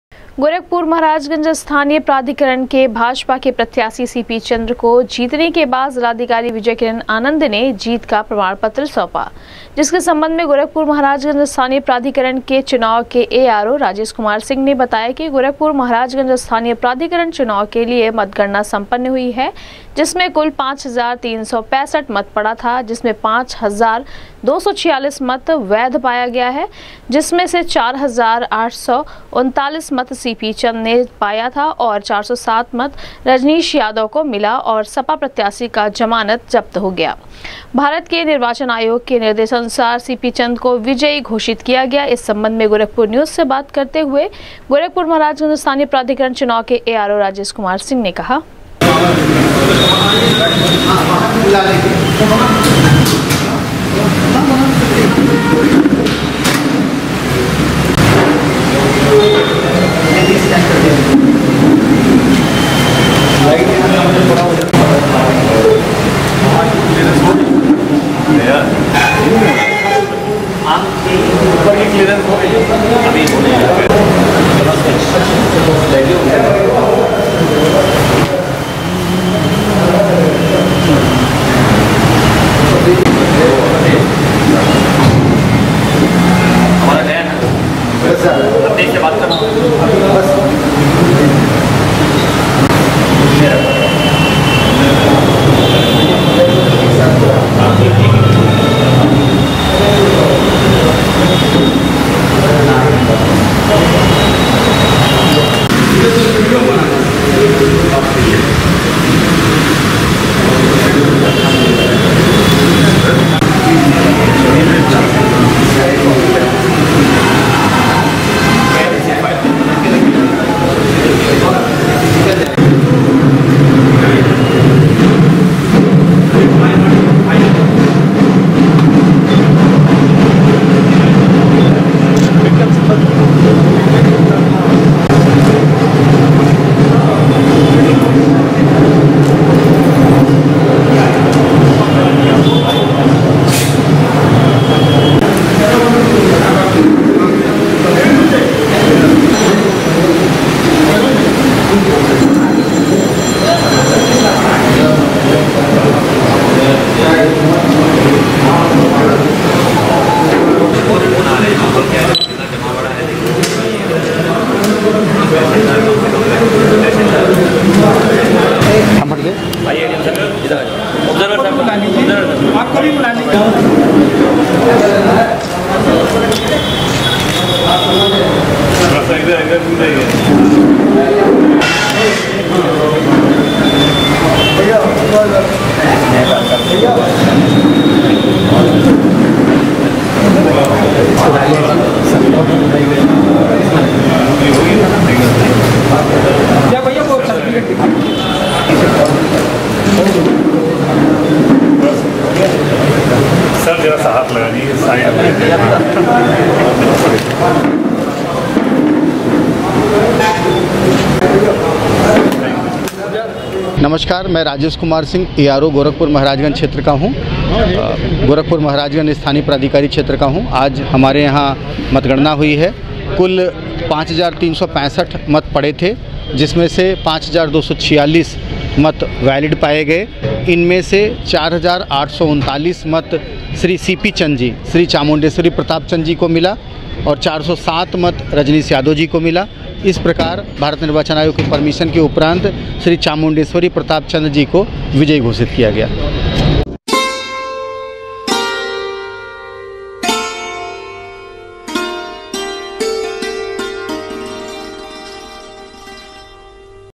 भाजपा प्रत्याशी सीपी चंद 4,839 मत पाकर हुए विजयी, सपा प्रत्याशी रजनीश यादव को मिला कुल 407 वोट,
गोरखपुर-महराजगंज स्थानीय प्राधिकरण चुनाव में भाजपा प्रत्याशी सीपी चंद 4,839 मत पाकर हुए विजयी, सपा प्रत्याशी रजनीश यादव को मिला कुल 407 वोट, सपा प्रत्याशी का जमानत हुआ जप्त,जिलाधिकारी/जिलानिर्वाचन अधिकारी विजय किरन आनंद ने दिया सीपी चंद को जीत का प्रमाण पत्र।।